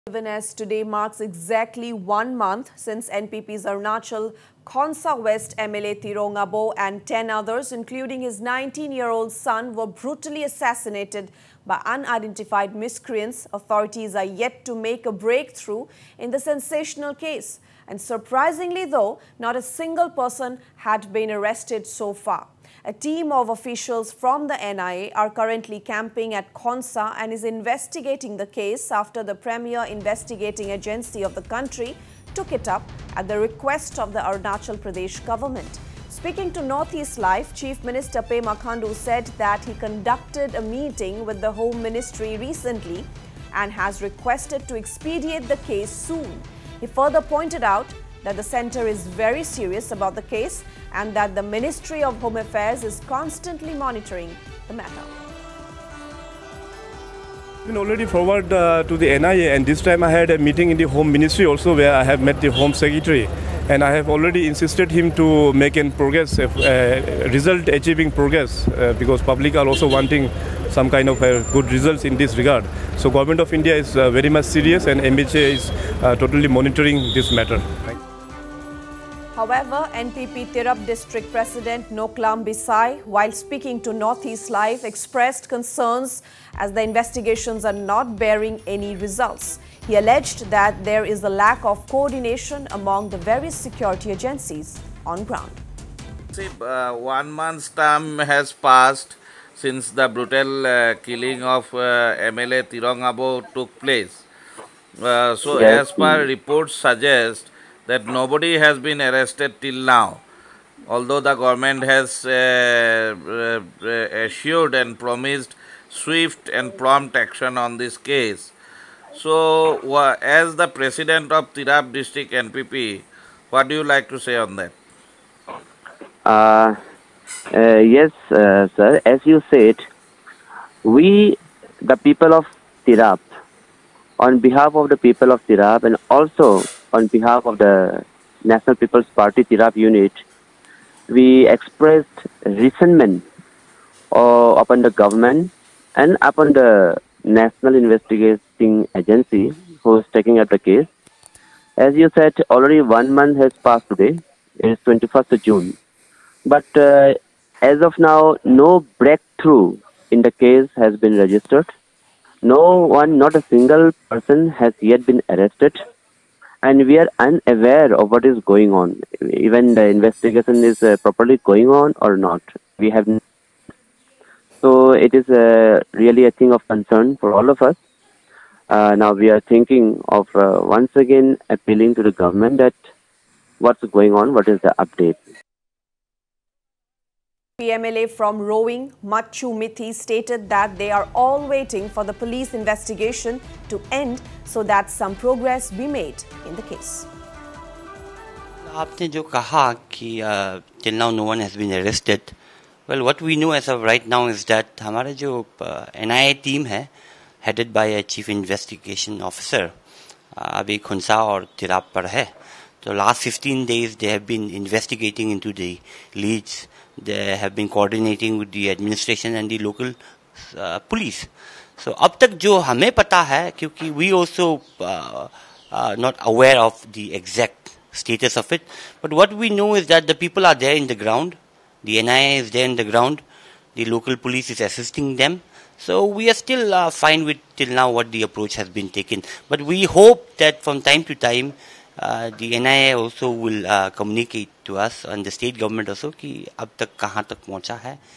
No breakthrough yet in Tirong Aboh assassination case
Even as today marks exactly one month since NPP's Arunachal Khonsa (West) MLA Tirong Aboh and ten others, including his 19-year-old son were brutally assassinated by unidentified miscreants, authorities are yet to make a breakthrough in the sensational case.
Speaking to Northeast Live, Chief Minister Pema Khandu said that he conducted a meeting with the Home Ministry recently and has requested to expedite the case soon.
However, NPP Tirap district President Noklam Bisai expressed concerns as the investigations are not bearing any results.
NPP MLA from Roing, Mutchu Mithi stated that they are all waiting for the police investigation to end so that some progress be made in the case.